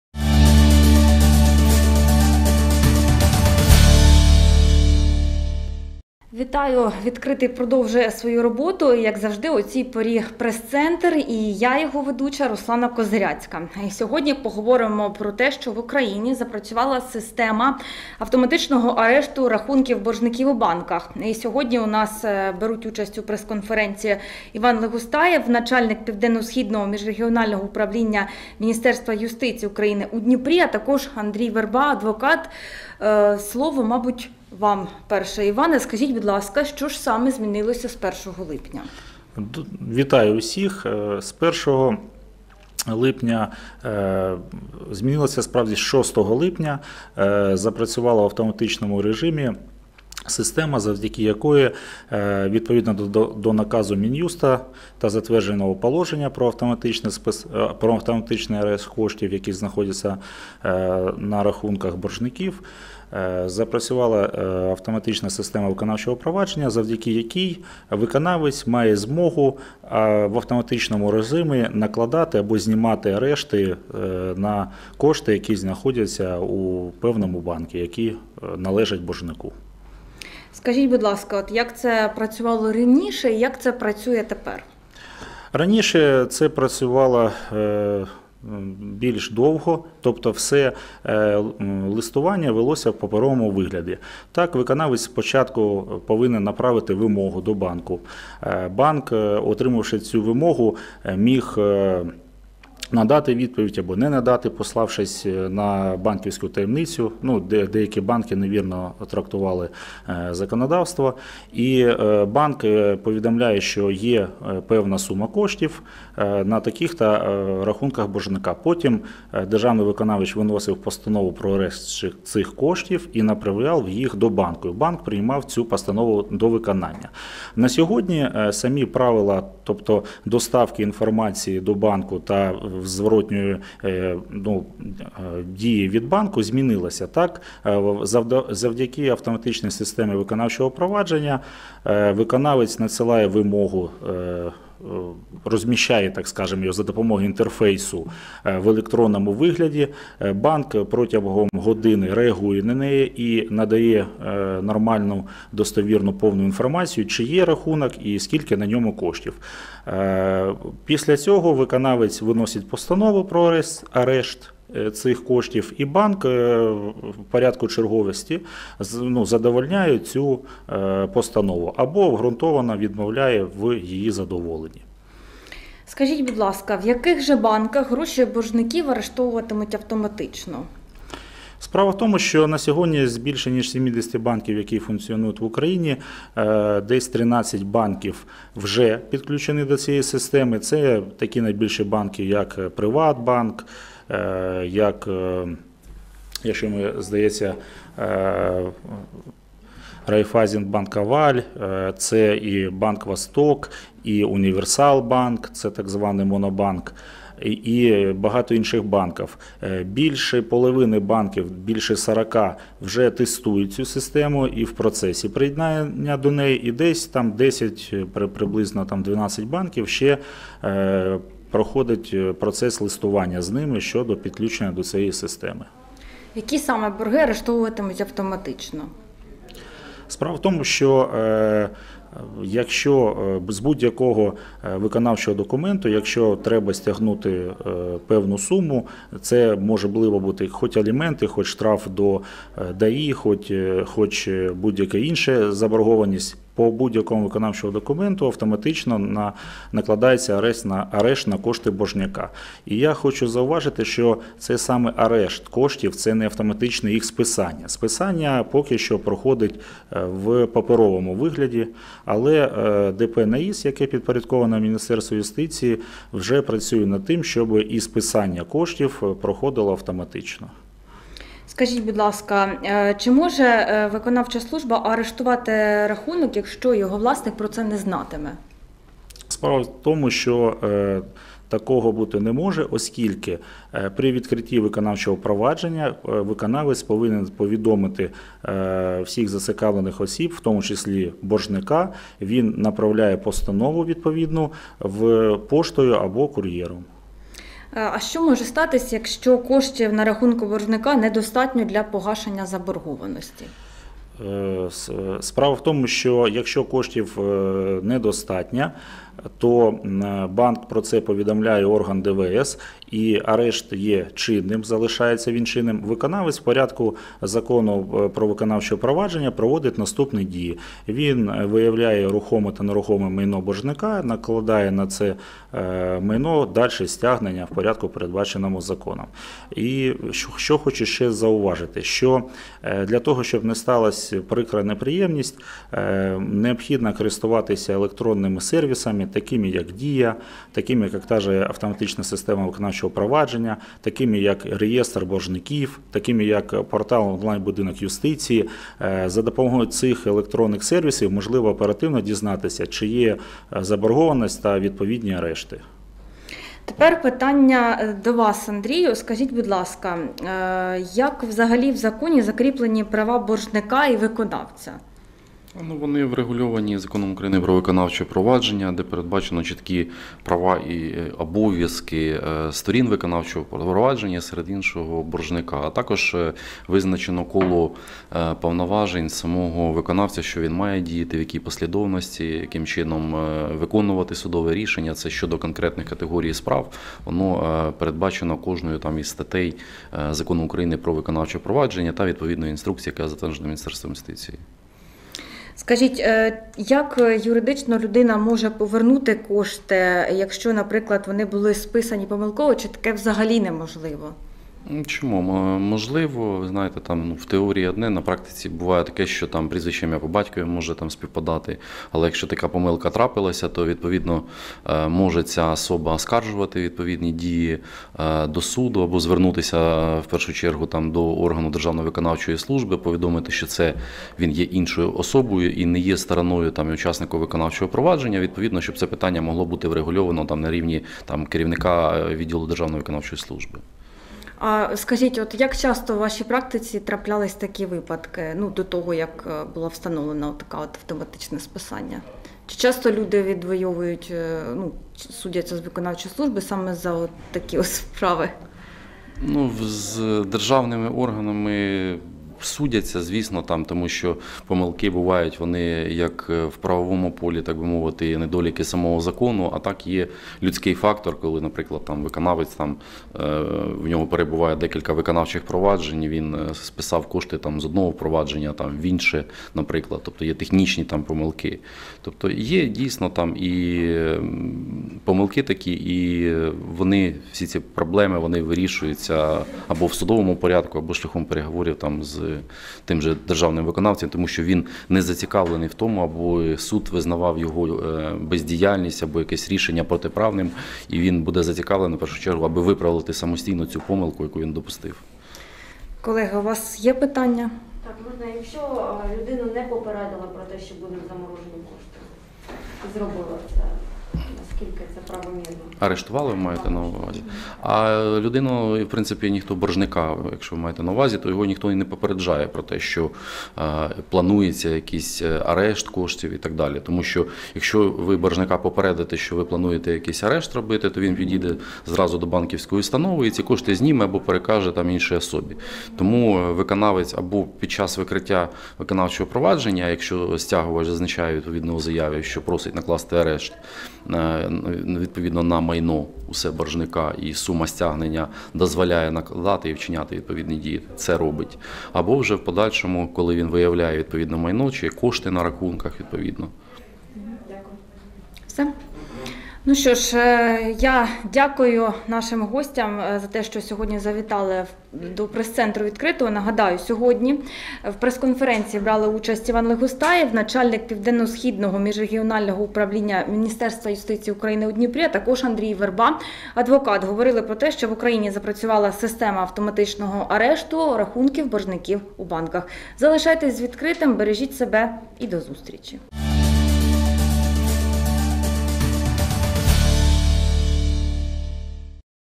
В Україні запрацювала система автоматичного арешту банківських рахунків боржників
Іван Легостаєв - начальник Південно-Східного міжрегіонального управління Мін'юсту України у Дніпрі

Андрій Верба - адвокат______________________________________________